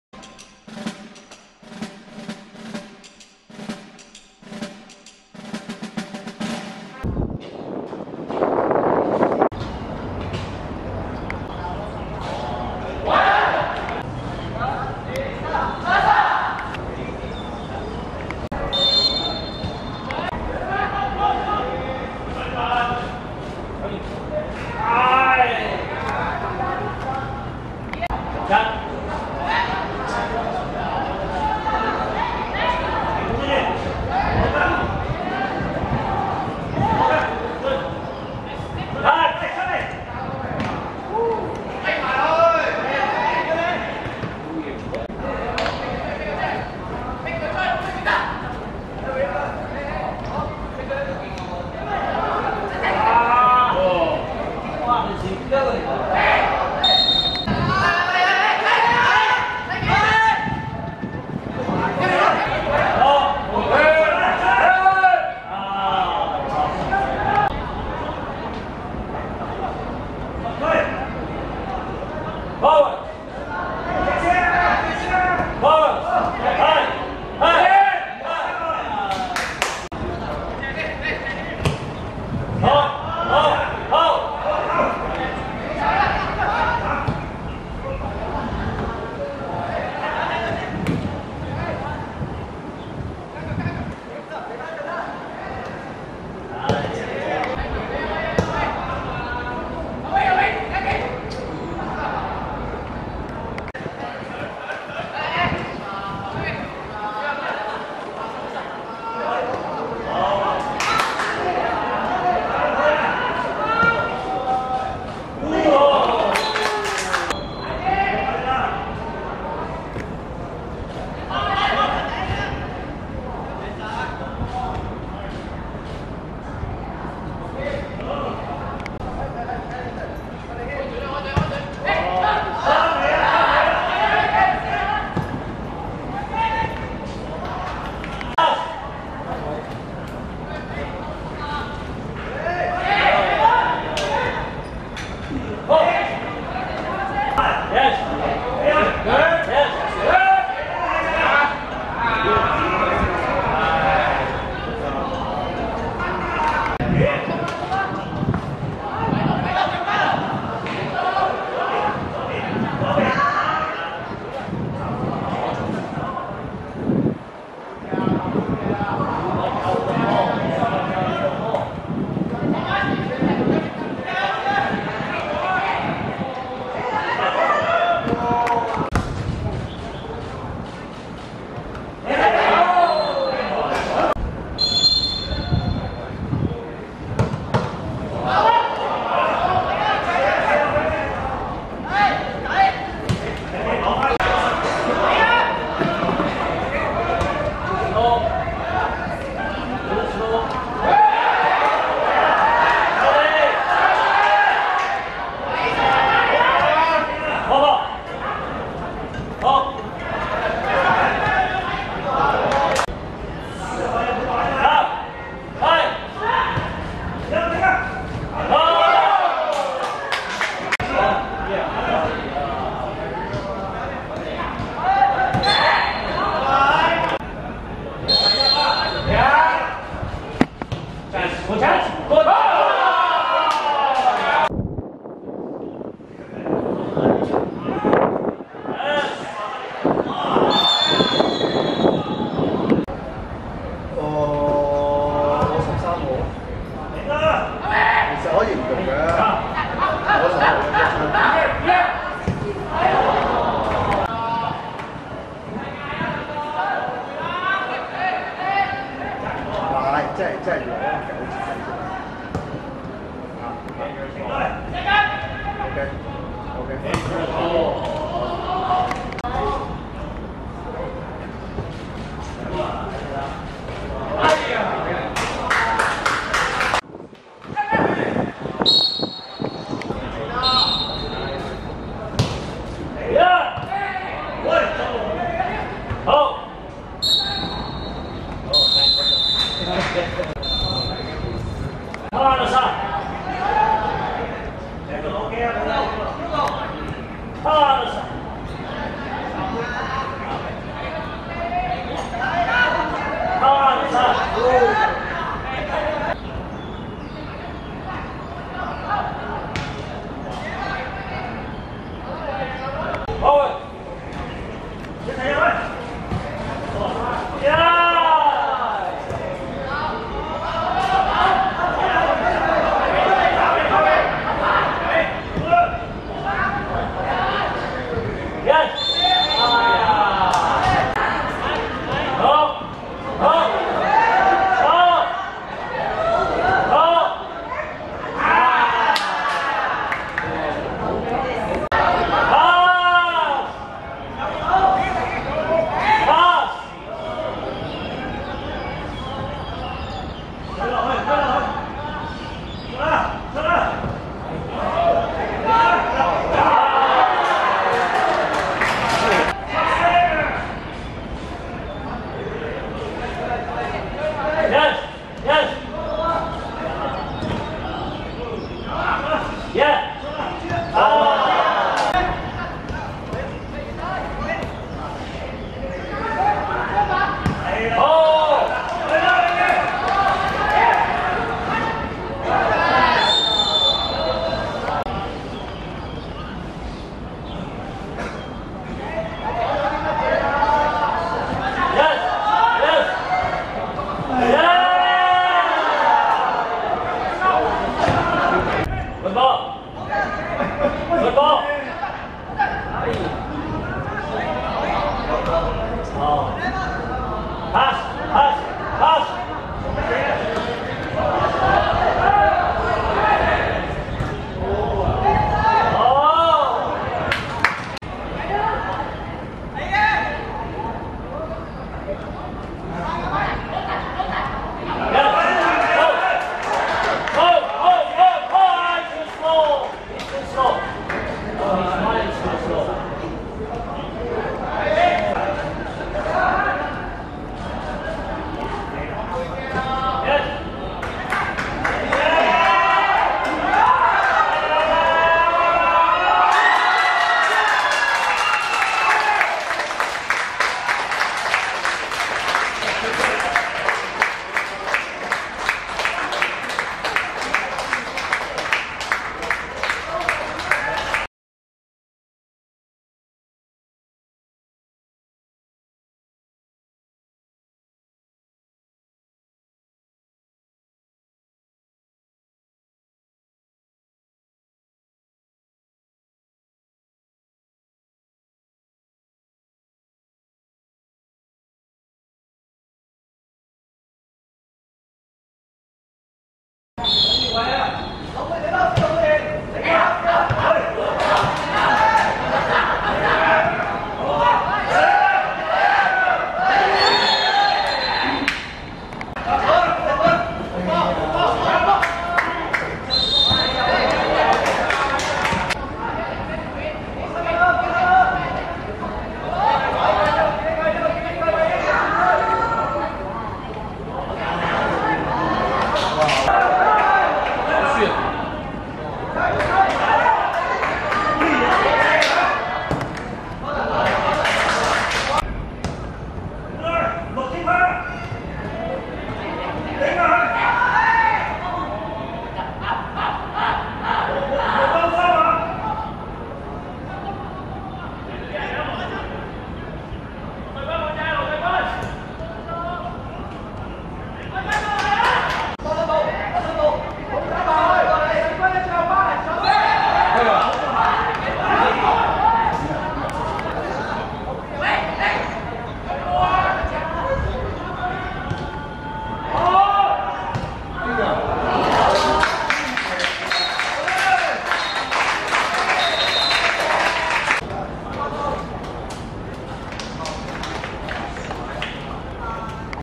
香港華仁vs喇沙(2017.10.4.D1學界足球甲組)精華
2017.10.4.蒲崗村仿真草塲 15:45 香港華仁(藍) 0 : 1 喇沙(白)